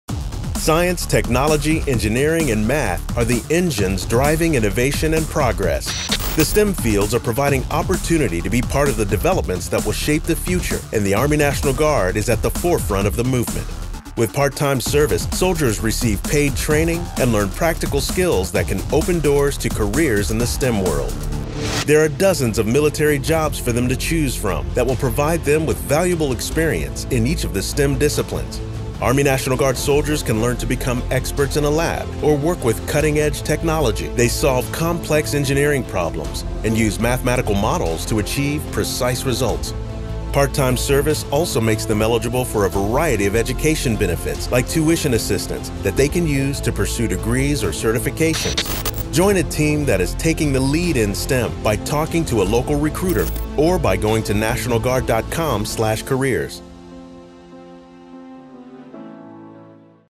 Army National Guard STEM 60 Second - SRSC
🧪️ Science, technology, engineering, and mathematics are driving innovation and progress. And the Army National Guard is on the front lines of STEM developments.

With dozens of part-time service opportunities in the STEM fields, the Army National Guard can help jump start your career.

💯 Join a team that is taking the lead in STEM by talking to a local recruiter, or by going

Video produced by SRSC (Strength Readiness Support Center)